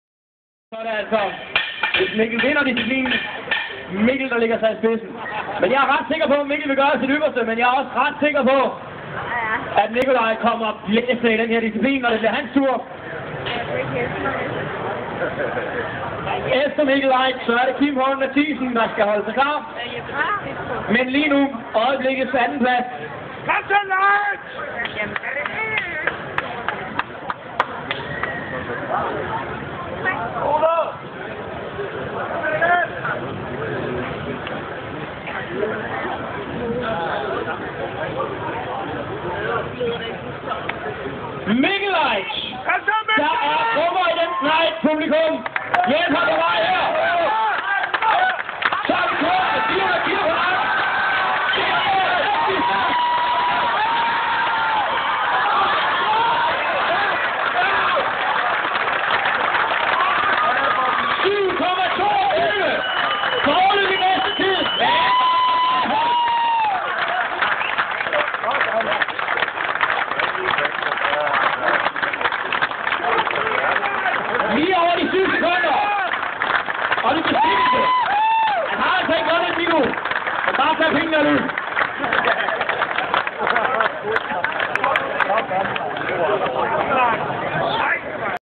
DSM 2010 - Mikkel Leicht part 4
Mikkel Leicht viser kræfter på dyrehavs bakken til Danmarks stærkeste mand 2010
For en god ordens skyld, skal det siges, Mikkel fik en anden plads.